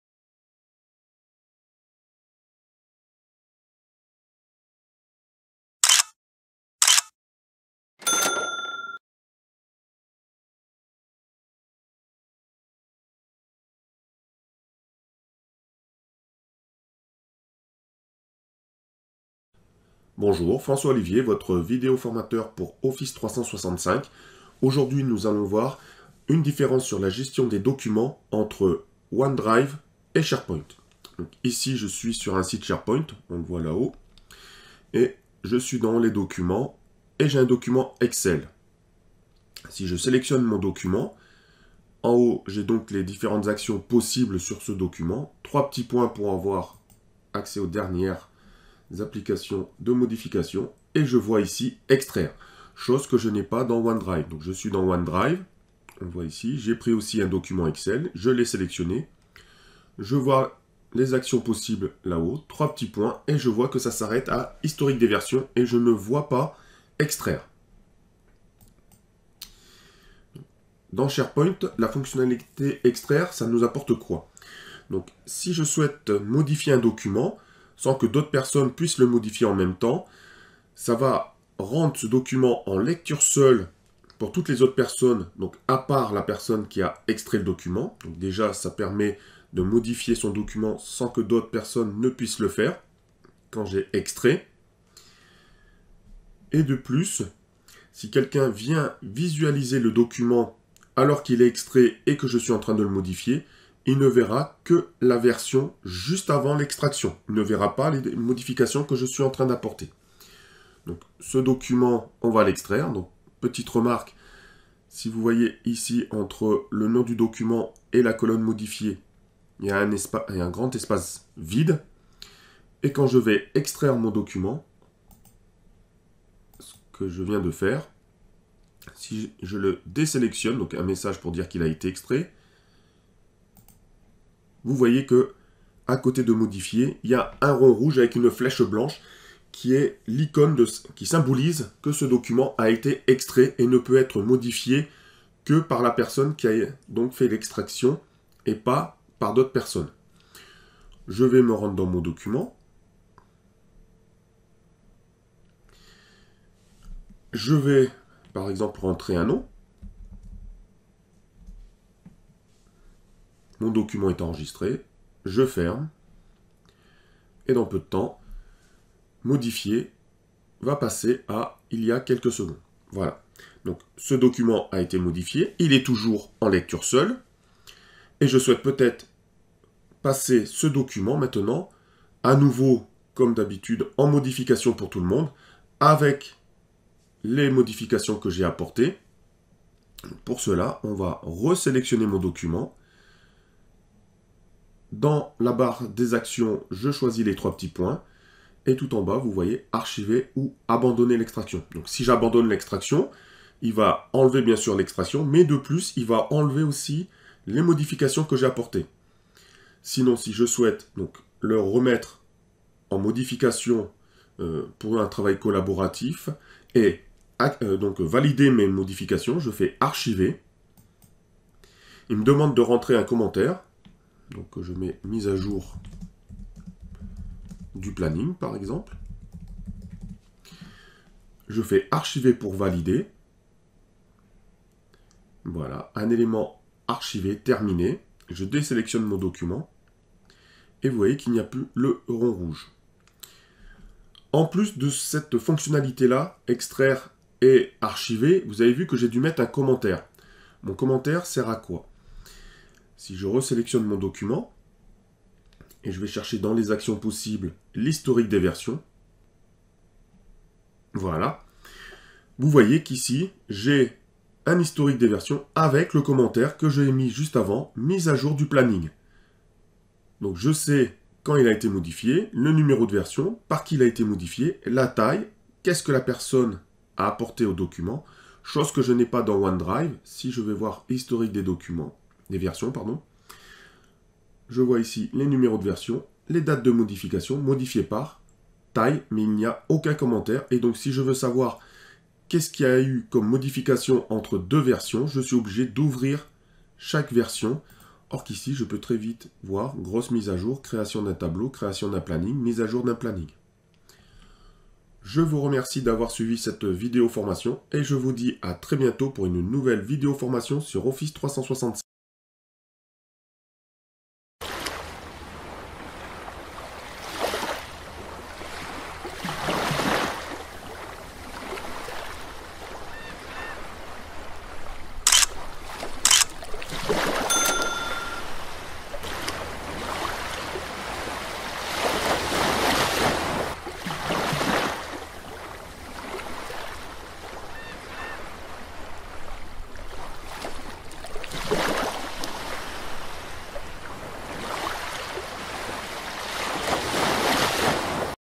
Formation SharePoint Extraire
Voici encore une différence dans la gestion des documents entre SharePoint et OneDrive : Extraire un document.
François Olivier, Vidéo Formateur pour Excel.